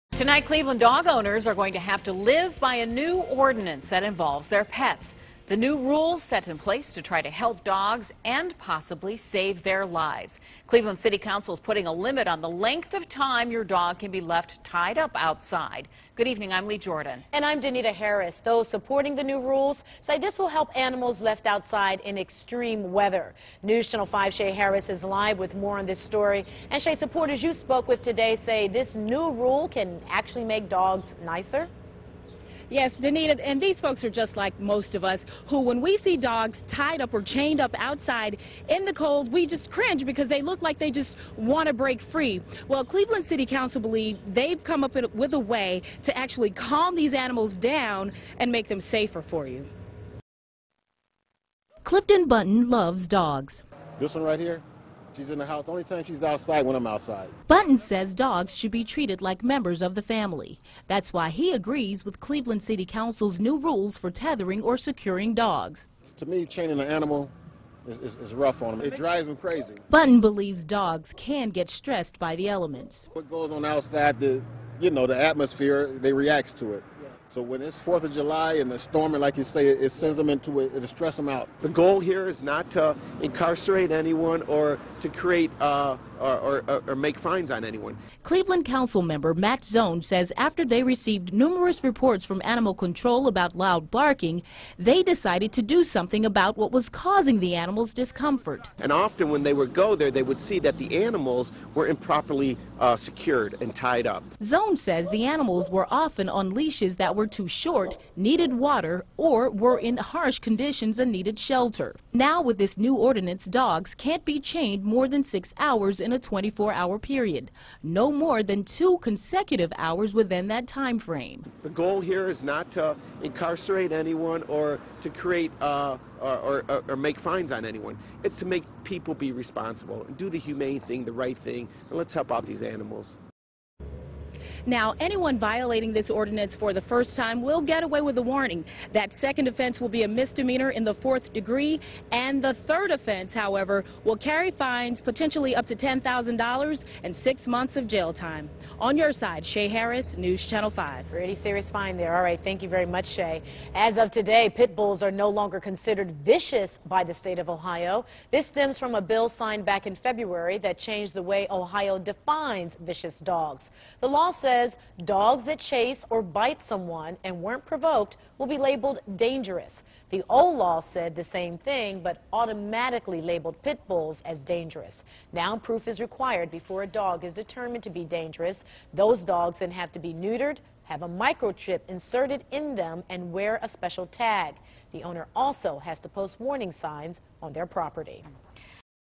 New dog ordinance
Pit bulls will no longer be labeled as "vicious" dogs under a new Ohio law.